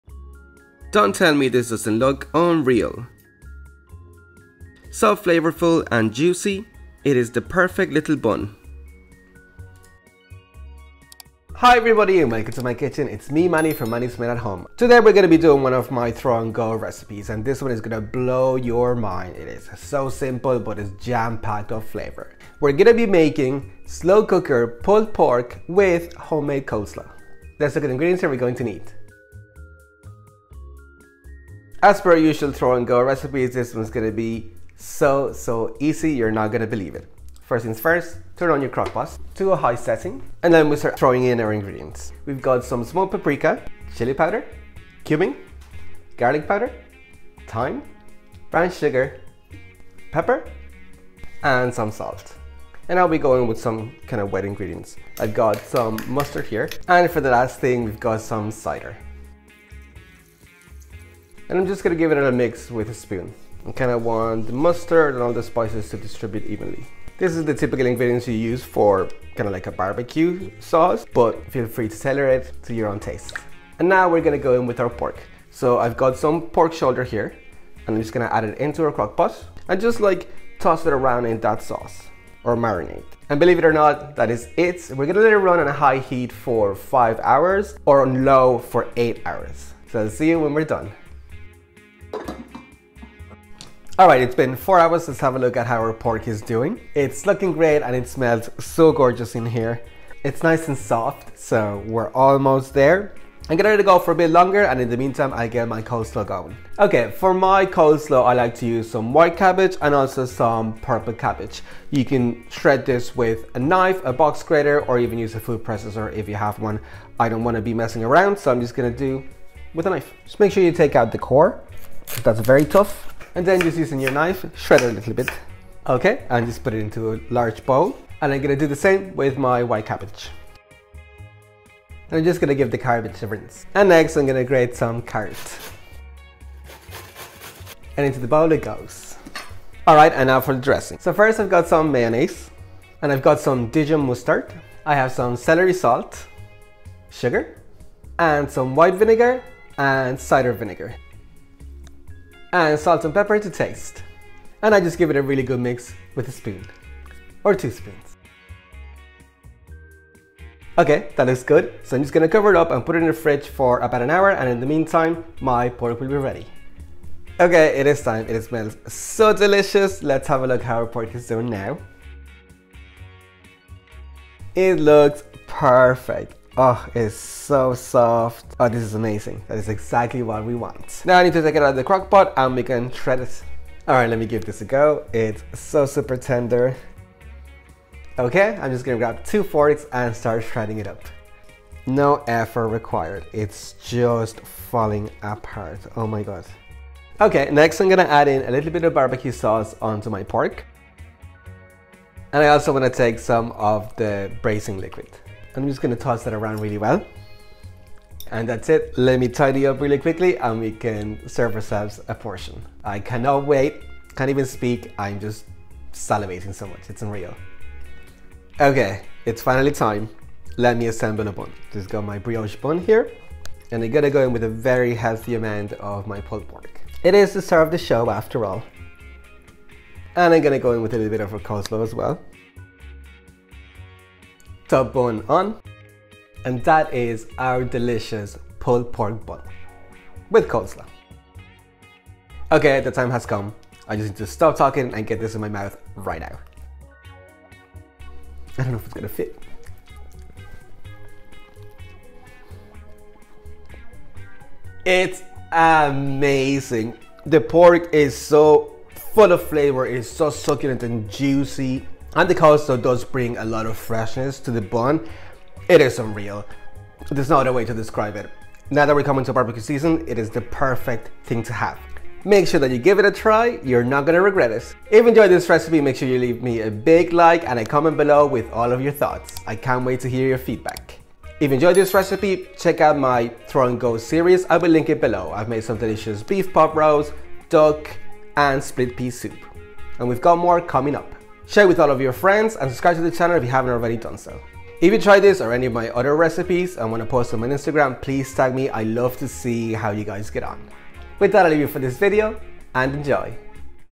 SLOW COOKER Pulled Pork & Coleslaw | Throw and Go
Hi everybody and welcome to my kitchen! Today I bring you another of my Throw and Go slow cooker recipes! It is almost BBQ season, but for those days when the weather doesnt want to cooperate we are making some Pulled Pork using our slow cooker, in a bun with some homemade coleslaw! It doesn't get much better than this!
Chapter:
0:00 Intro
0:27 Ingredients
0:40 Marinade
1:46 Coleslaw
2:46 Shredding Pork
3:38 Assembling bun
4:02 Tasting
4:36 Outro

Ingredients:
Soft Brioche Bun
Pulled Pork -
2 Tsp Garlic Powder
2 Tsp Smoked Paprika
2 Tsp Cumin
2 Tsp Pepper
2 Tsp Thyme
2 Tsp Brown Sugar
2 Tsp Mustard
1 Tsp Salt
1 Tsp Chilli Powder
250ml Apple Cider
2kg Pork Shoulder
250ml BBQ Sauce

Coleslaw -
3 Cup White Cabbage
2 Cup Red Cabbage
1 Cup Carrot
1/2 Cup Mayo
1 Tbsp Dijon Mustard
1/2 Tsp Celery Salt
2 Tsp Sugar
2 Tbsp White Vinegar
1/2 Tbsp Apple Cider Vinegar